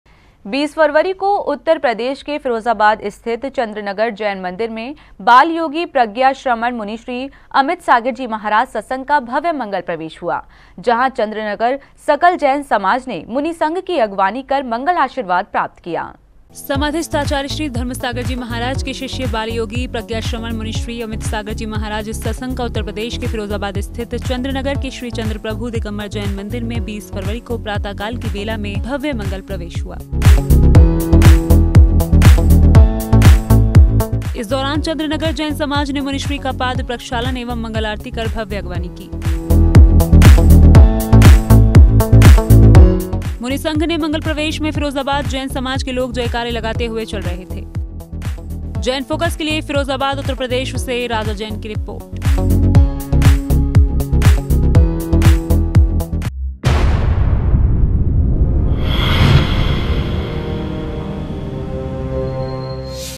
Firozabad News: मुनिश्री अमितसागर जी महाराज ससंघ पहुंचे फिरोजाबाद | JAIN FOCUS
मुनिश्री अमितसागर जी महाराज ससंघ पहुंचे फिरोजाबाद
20 फरवरी को चन्द्रनगर जैन मन्दिर में हुआ पदार्पण
भक्तों ने मुनिश्री का पाद प्रक्षालन कर लिया आशीर्वाद